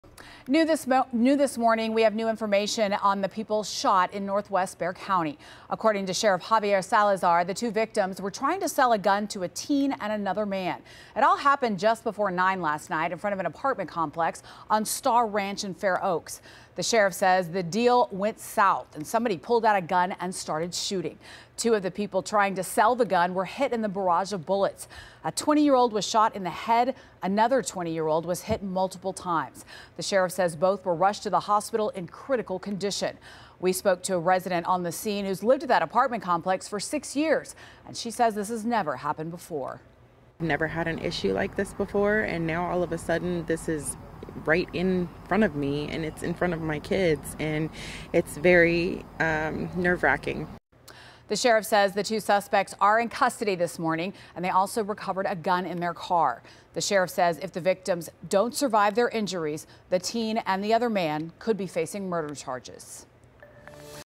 Two people left in critical condition after trying to sell a gun, BCSO says
One of the victims called deputies to report they had been shot and the other has been taken to local hospital with serious injuries, BCSO says.